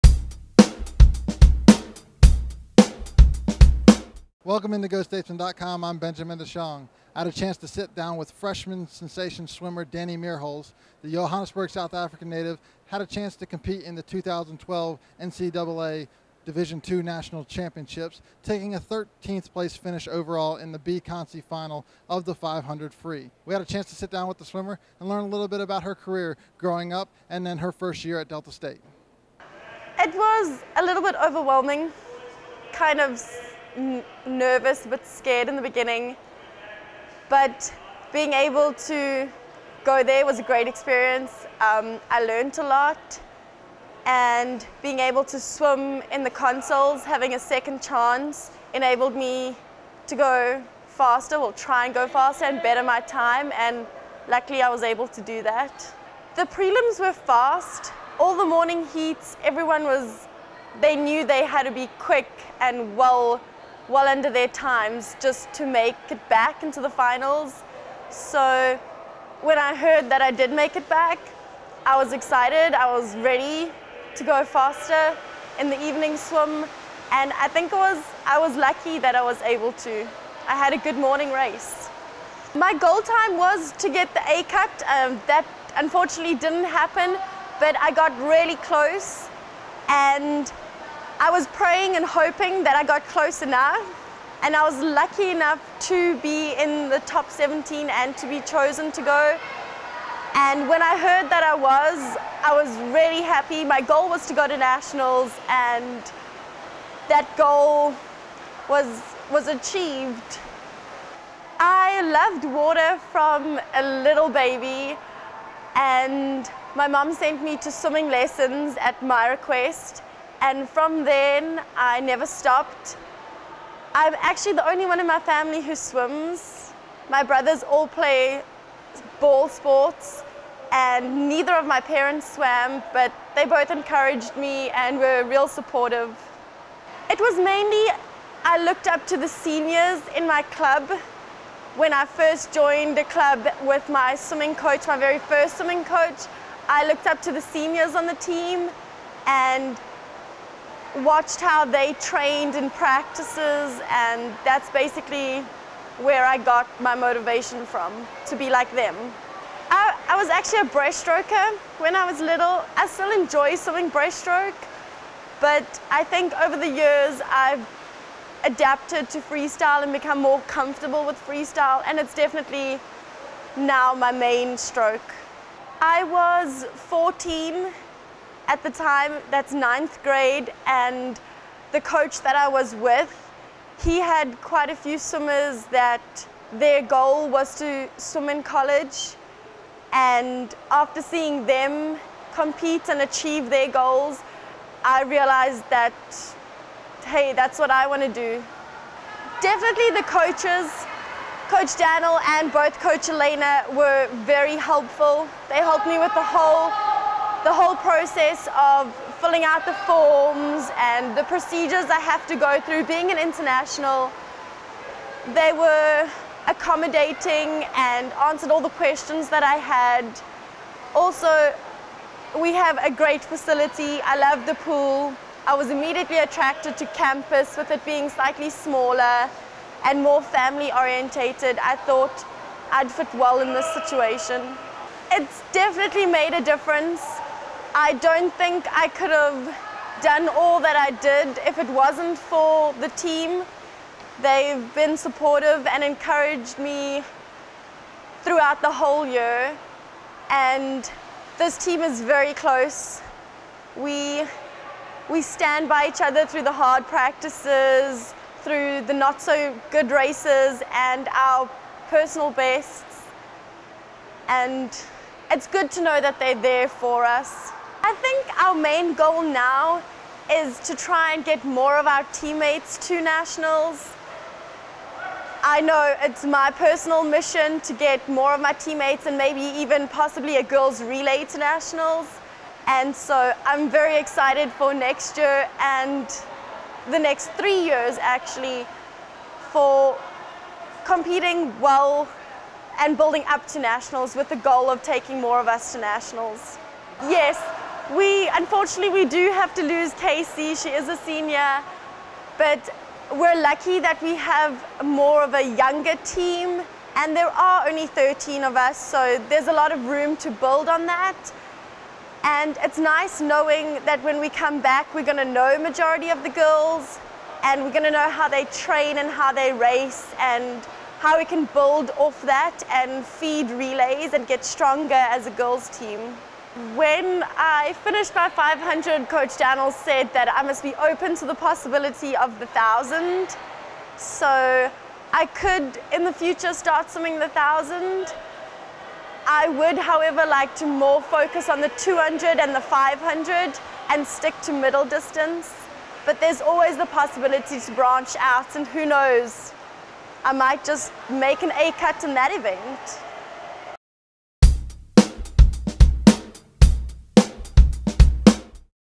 10 for 10 with DSU's Dani Meerholz: Reflections from year one
Johannesburg, South Africa native Dani Meerholz recently completed her first year at Delta State helping the swimming and diving team to high marks. Meerholz also had an outstanding season individually which saw her compete in the NCAA Division 2 Nationals in Mansfield, Texas. She caught up with GoStatesmen.com for an in-depth Q&A about her freshman year.